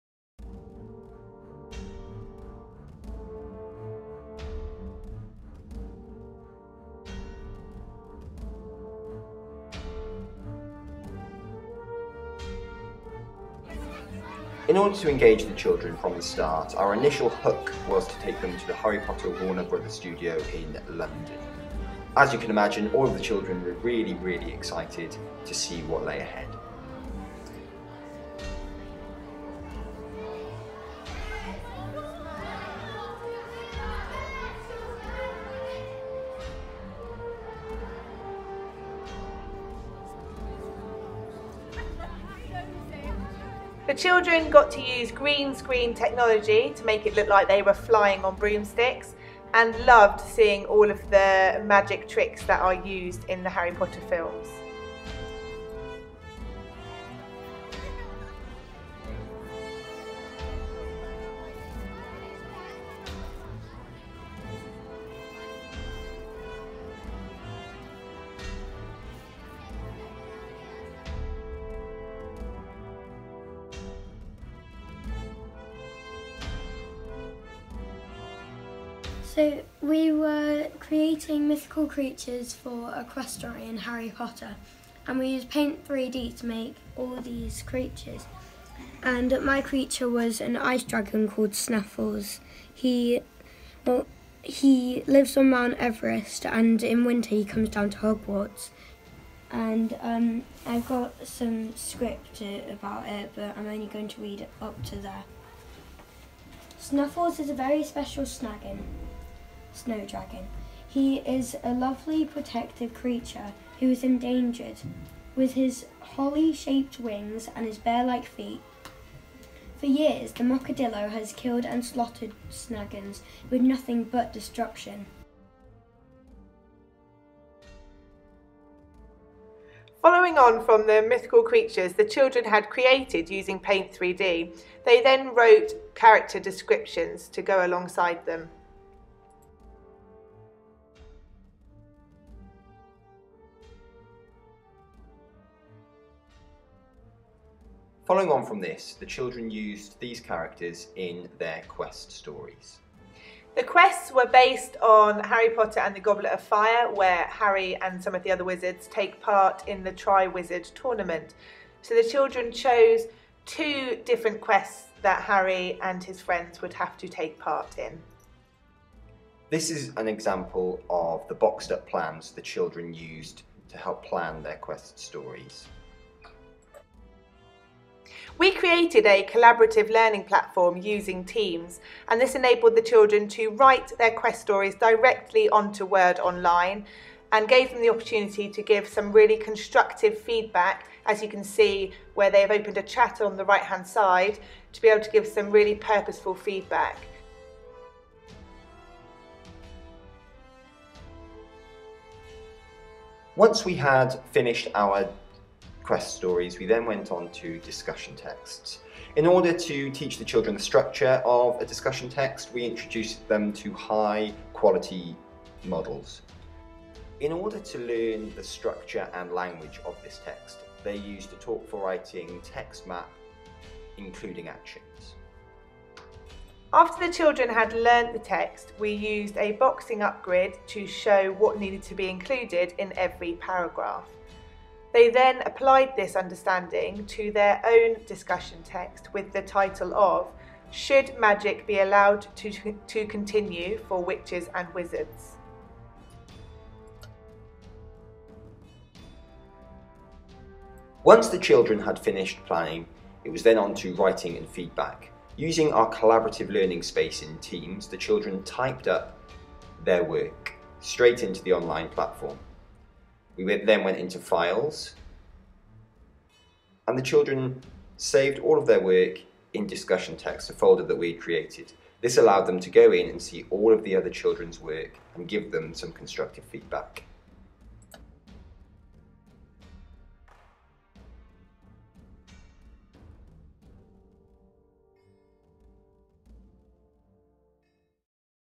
Year 6 Harry Potter English
This video demonstrates how we used Harry Potter as a hook to engage children's writing.  It also explains how we incorporated both Microsoft Teams and Paint 3D to enhance their learning.